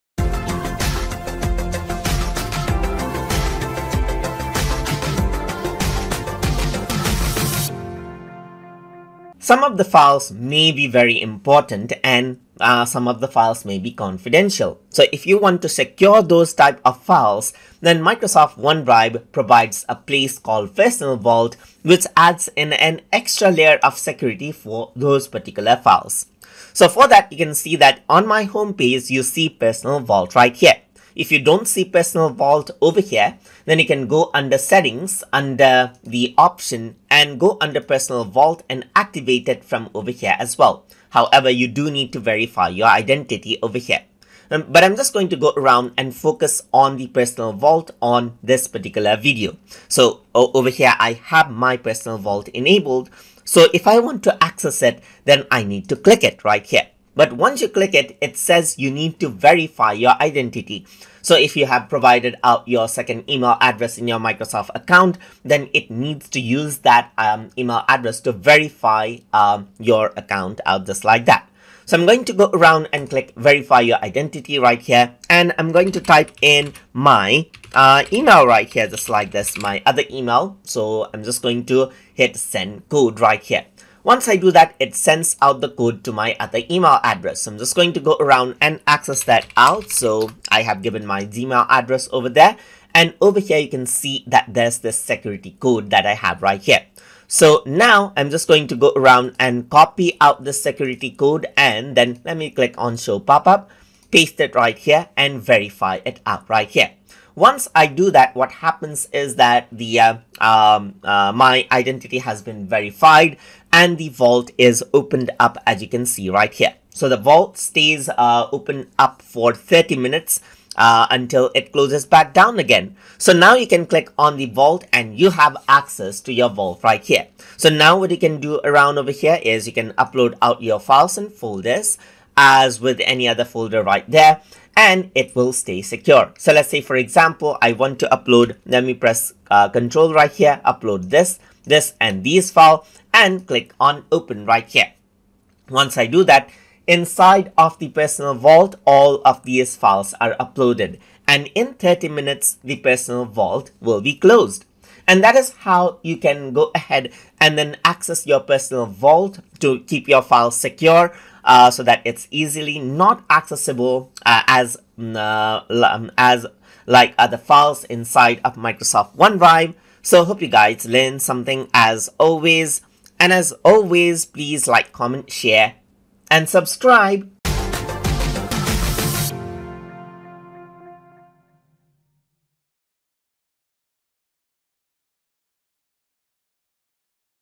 Microsoft OneDrive - Lesson 5 - Enabling and Accessing Personal Vault
In this tutorial, we will be discussing about Enabling and Accessing Personal Vault in Microsoft OneDrive

Learn the basics of using Microsof OneDrive including how to upload, download, backup, sync files, covert and manage files.

This lesson has been made using Microsoft OneDrive using Chrome Browser and Microsoft OneDrive for Windows but is applicable for Mac Version of OneDrive as well.

If you like this video, here's our entire playlist of Microsoft OneDrive tutorials: